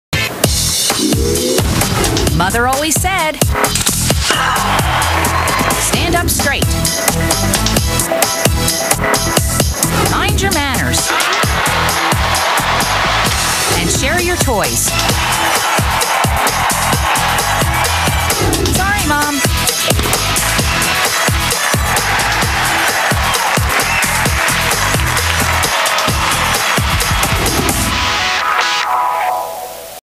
WTA Commericial Mother Always Said
just an old commerical from last year that I found while looking at an old match I recorded...